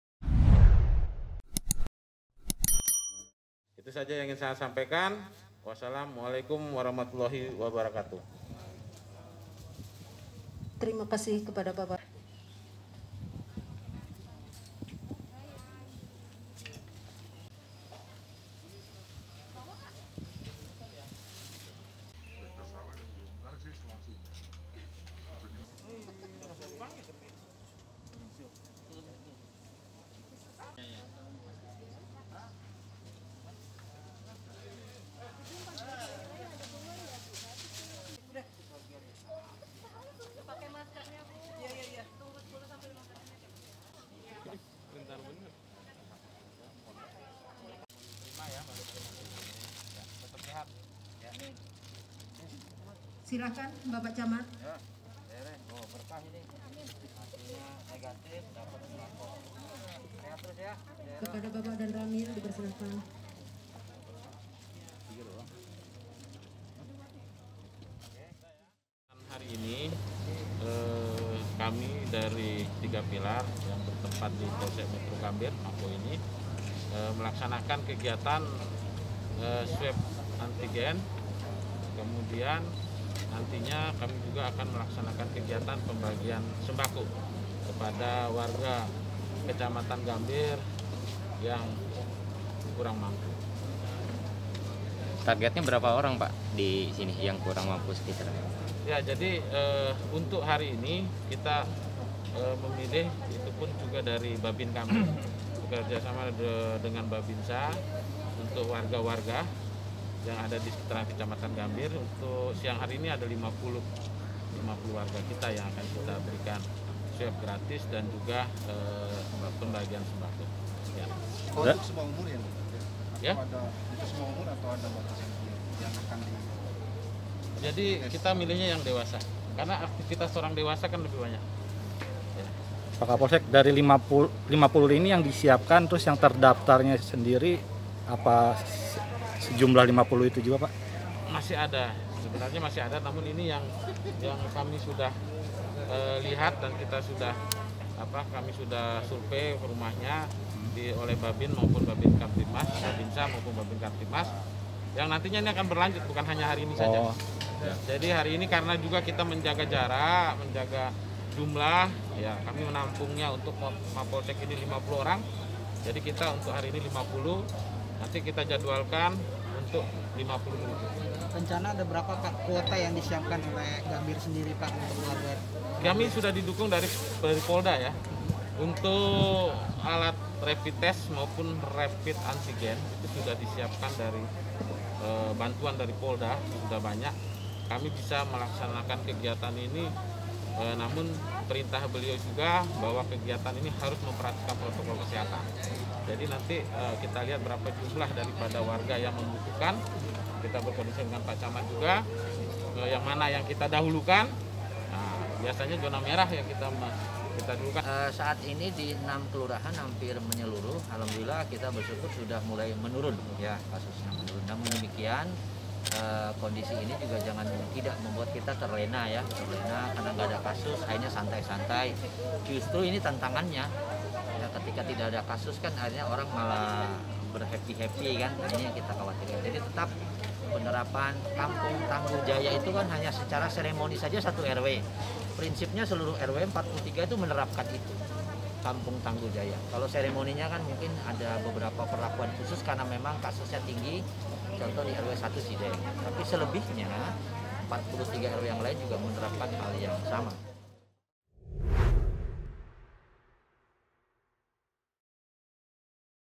GAMBIR, PULUHAN WARGA BERADA DI ZONA MERAH POLSEK GAMBIR LAKUKAN RAPID TEST ANTIGEN
JAKARTA, POSKOTA.CO.ID - Puluhan warga yang berada di zona merah wilayah Kecamatan  Gambir, Jakarta Pusat, mengikuti tes rapid antigen di halaman Mapolsek Metro Gambir, Selasa (2/2/2021).

Kegiatan yang didahului dengan pemeriksaan riwayat kesehatan terlebih dulu, dilakukan secara tertib dan memperhatikan protokol kesehatan Covid-19.

Kapolsek Gambir, AKBP Kadek Budiarta mengatakan, sebanyak 50 warga yang mengikuti tes rapid antigen. Kegiatan, untuk mendeteksi dini sekaligus pencegahan penularan Covid-19 yang ada di wilayahnya.